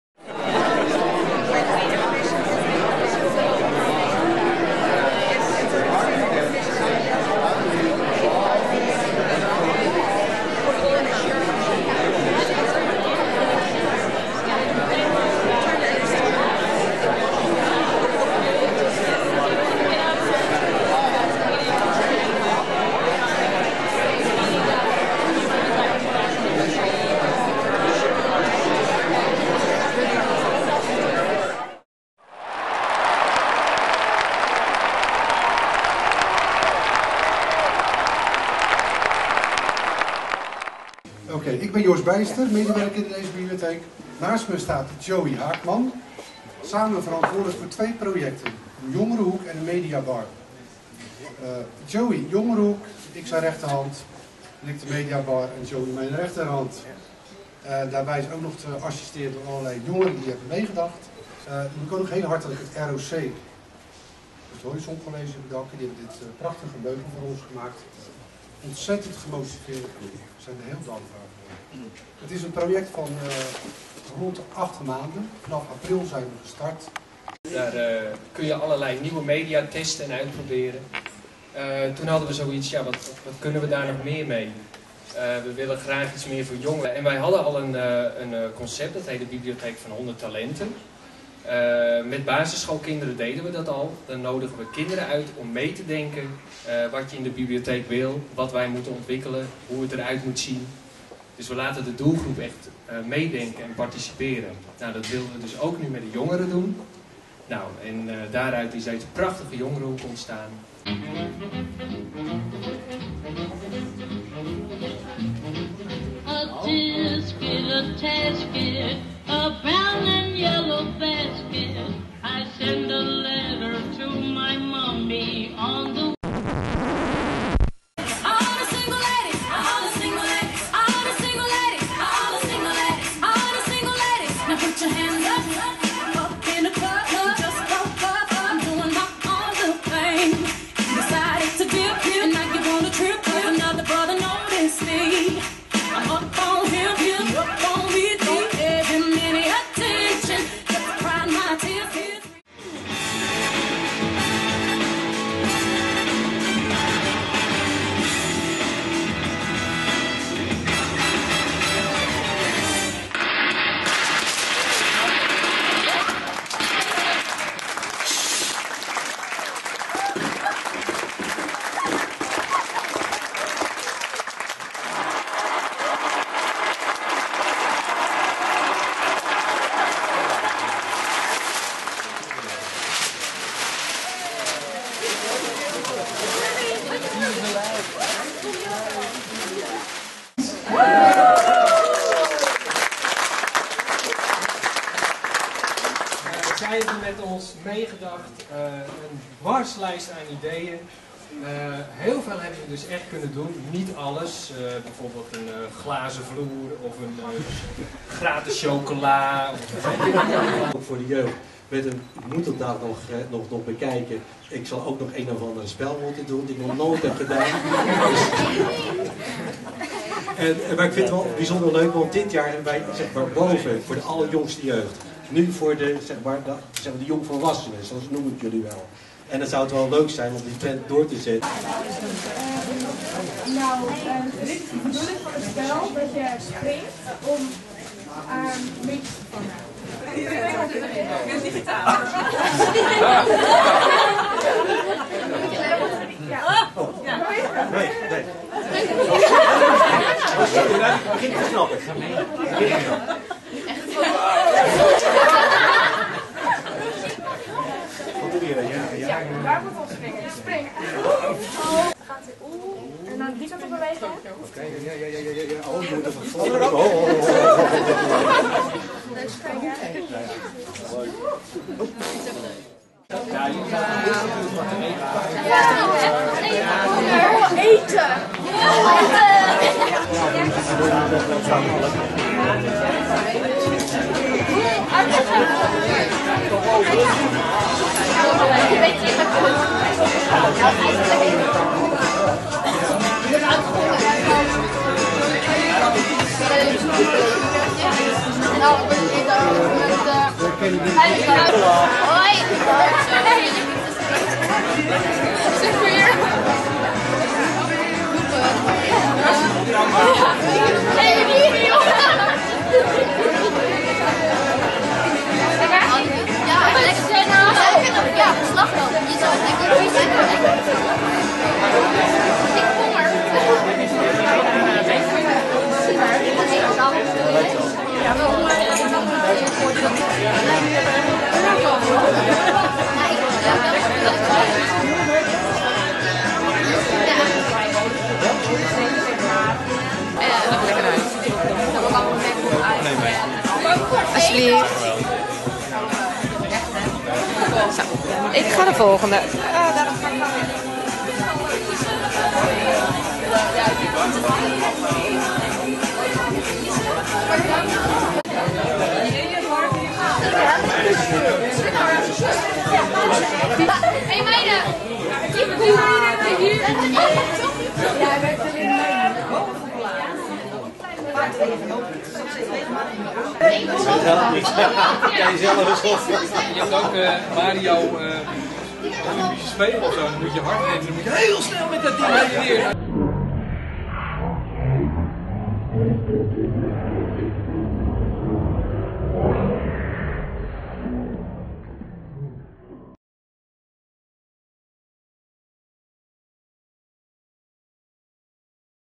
Opening Chill zone
opening jongerenhoek van Bibliotheek Kennemerwaard locatie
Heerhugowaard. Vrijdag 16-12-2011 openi